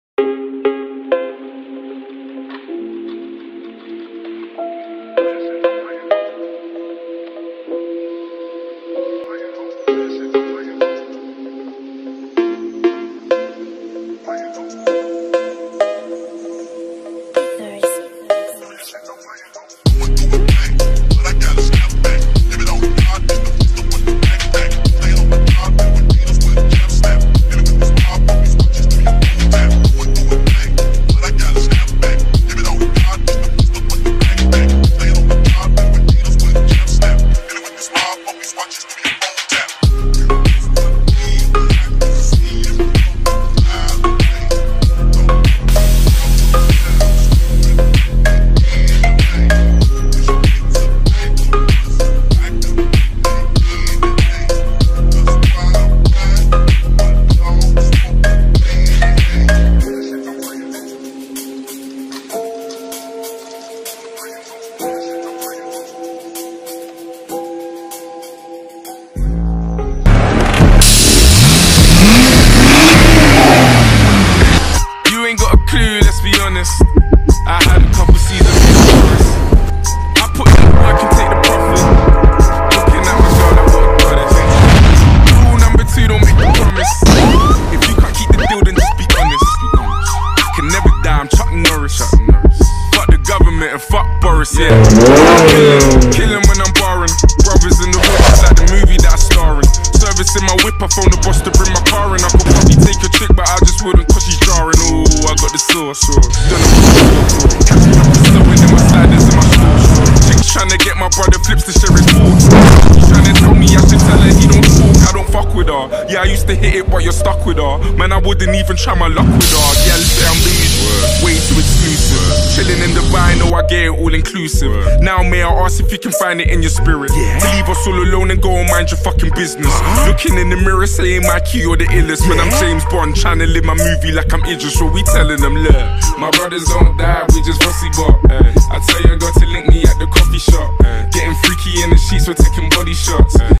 Wide Body E90 & Convertible E36 | KTFilms
Spent the day with these two beauties, Custom Wide Body E90 & Elegant E36. Both of these Rides are on Air Suspension and both from East London, South Africa. The Owner gave us a day with his two special "Girls"